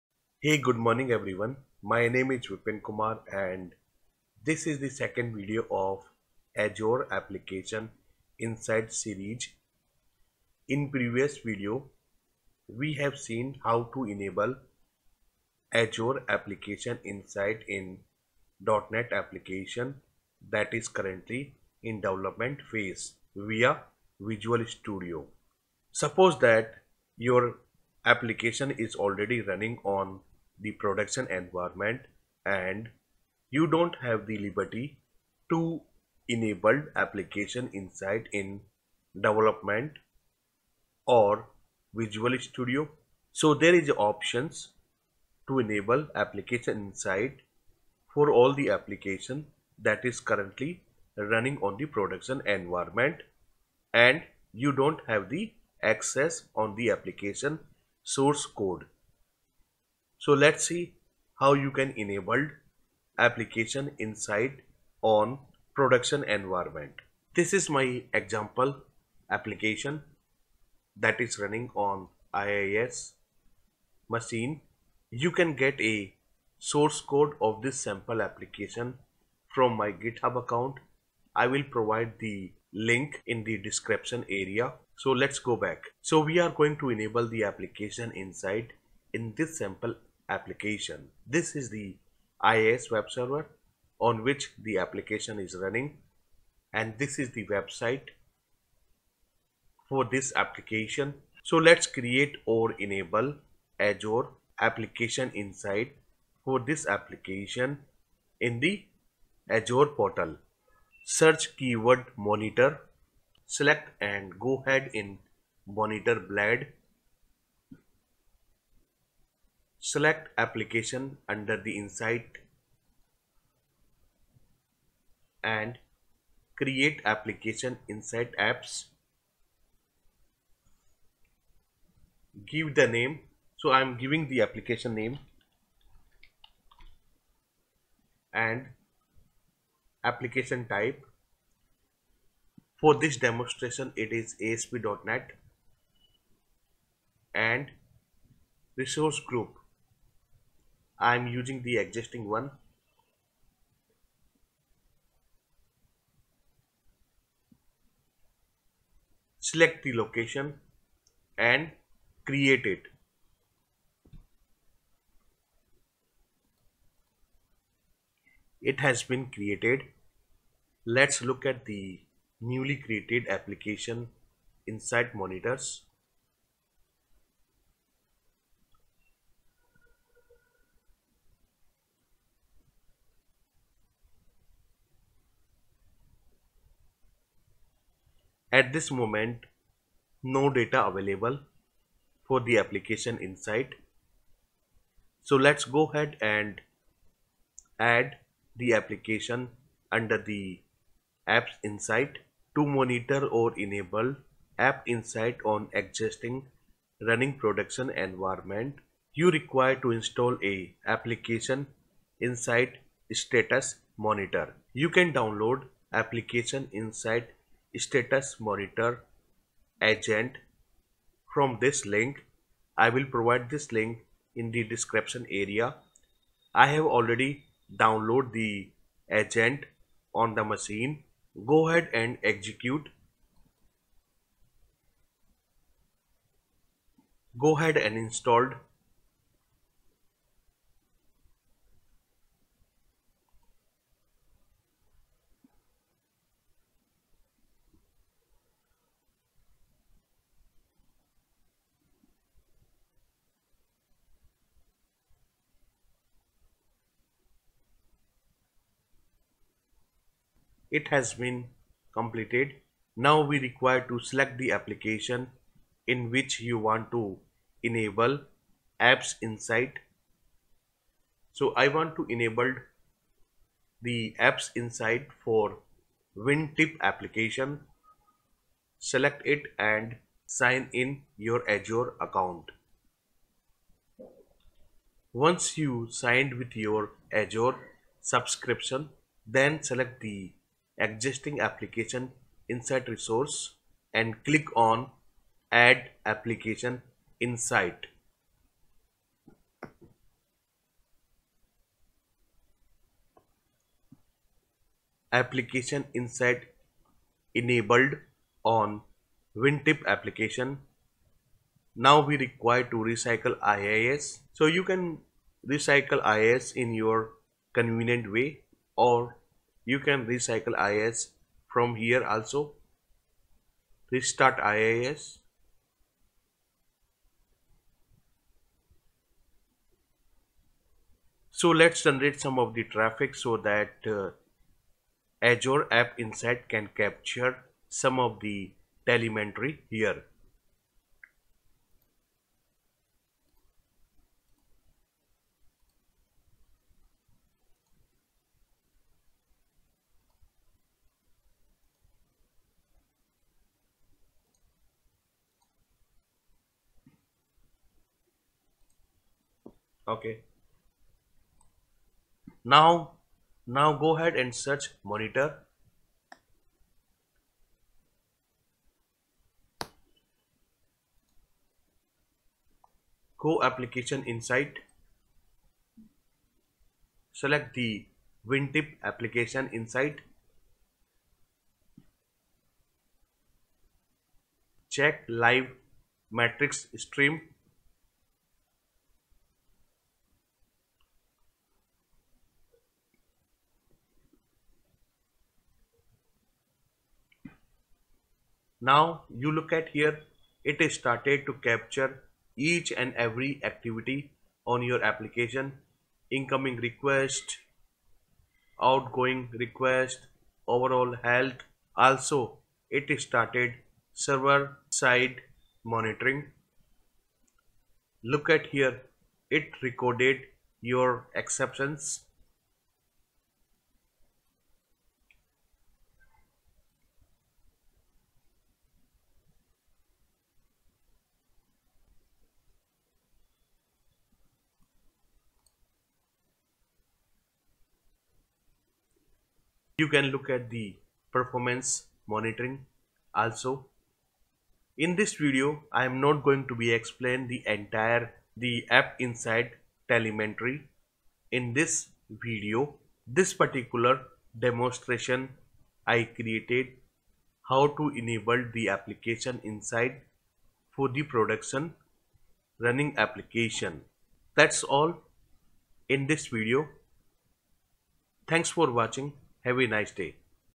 How to enable Azure Application insight monitor on production running application
1. How to install and configure Application Insights Status Monitor Agent.
2. Enabled Application Insights on Production Application.
3. Validation of APM for production app.